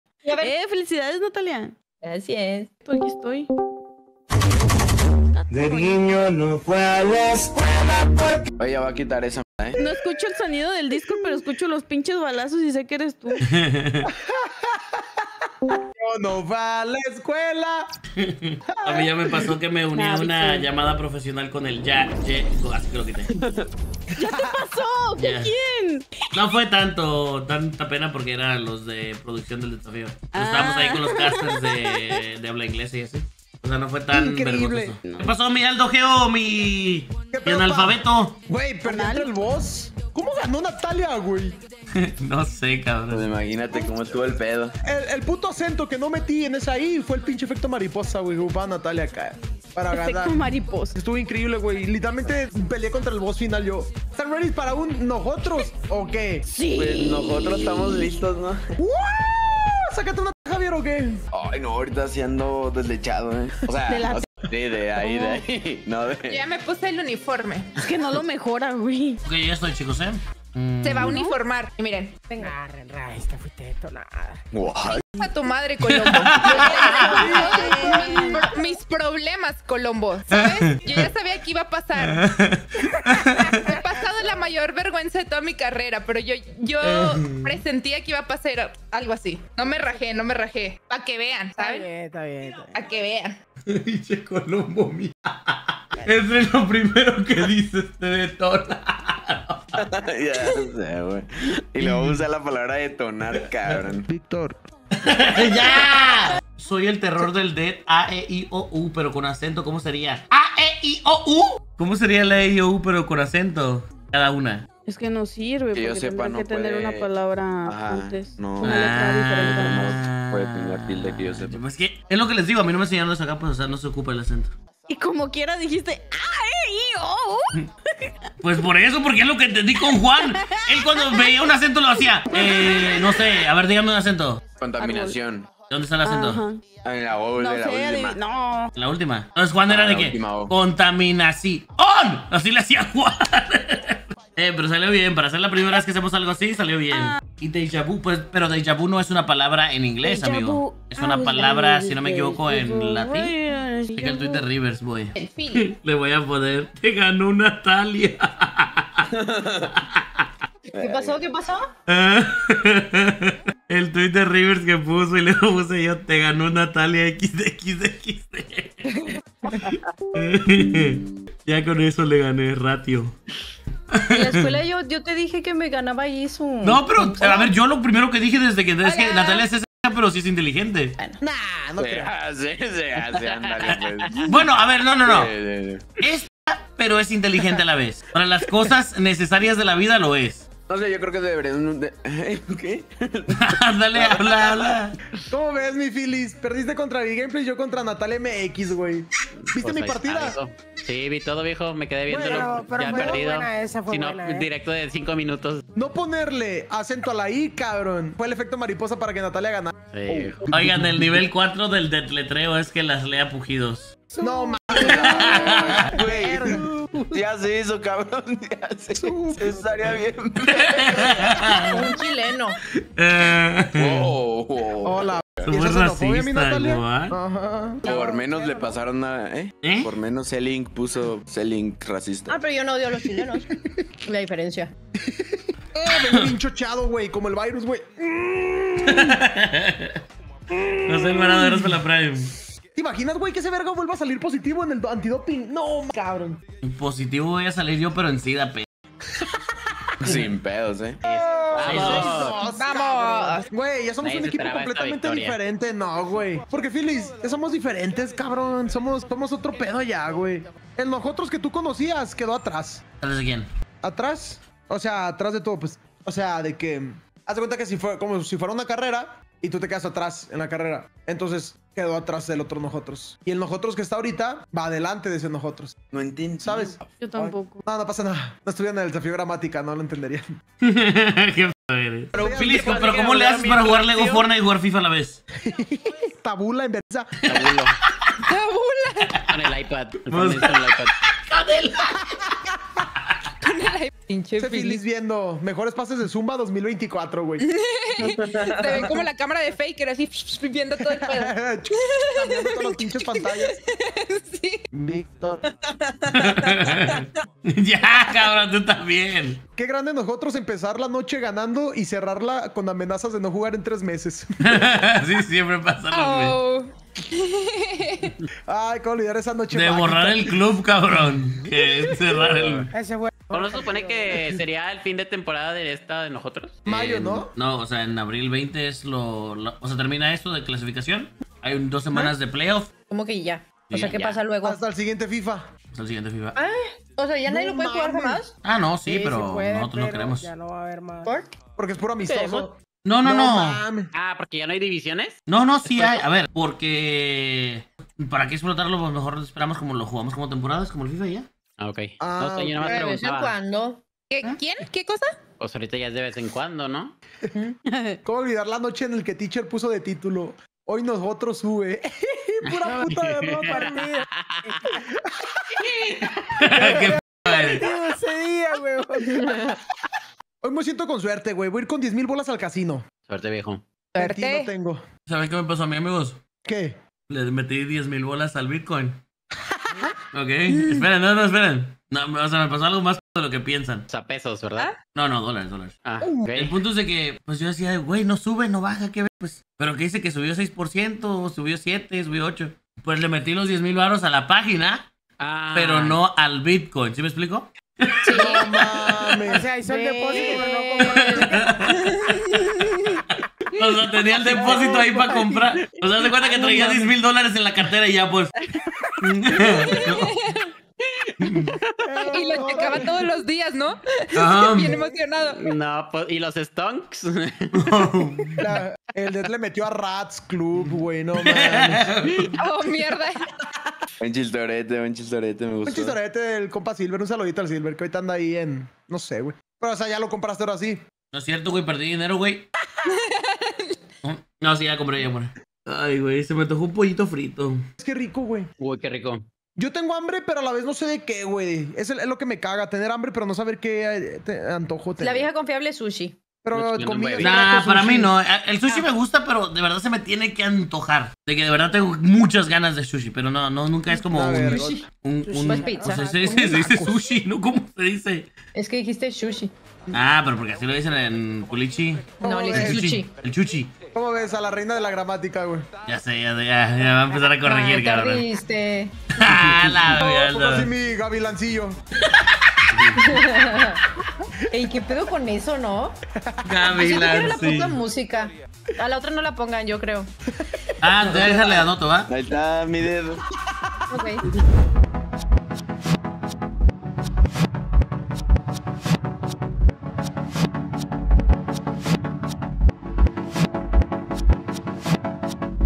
ELDED y ALDO MOLESTAN A NATALIA POR EL DEDLETREO
@deDOS  y @aldo_geo  MOLESTAN A @Nataliamx  POR GANAR EL DEDLETREO

en llamada @renrize @Nephtunie @Colomboanashe @JavierMtz @Filisgg